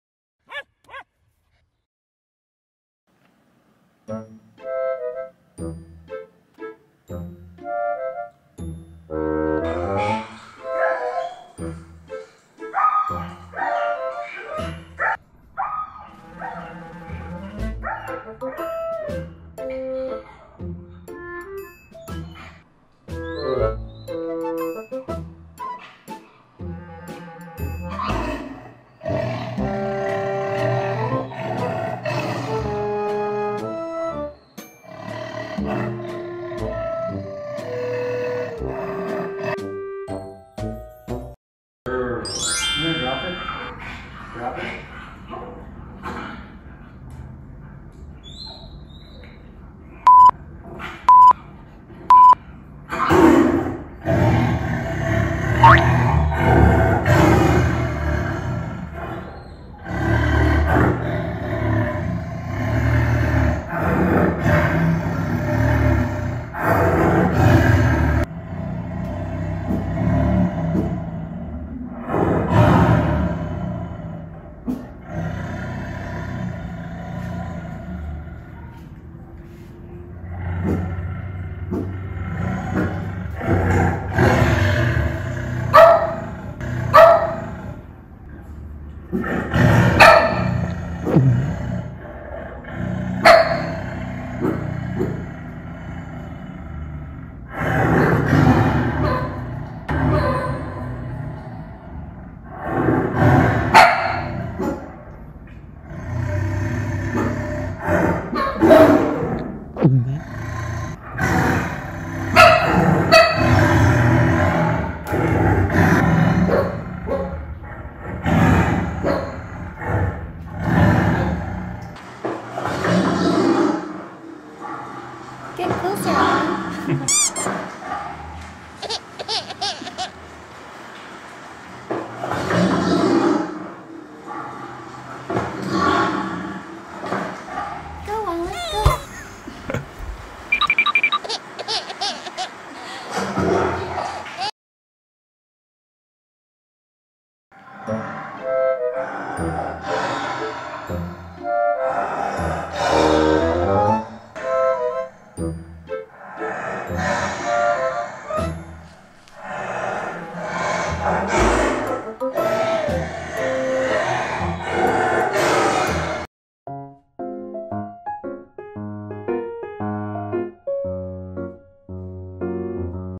Puppy Reaction To Tiger On TV
One morning, our cute little cavoodle puppy Olive was snuggled up on the couch with us while we were watching a nature documentary. Suddenly, a majestic tiger appeared on the screen, and we couldn't help but wonder how our dog would react to seeing such a big cat! 🐅 In this video, we captured our puppy's reaction to seeing a tiger on TV for the first time. Watch as our cute little pup jumps, barks, and tilts her head in confusion and excitement, all while trying to figure out what this strange creature on the screen is. Will puppy Olive be brave enough to get closer to the TV to investigate? You'll have to watch to find out! Don't miss this entertaining and heartwarming moment.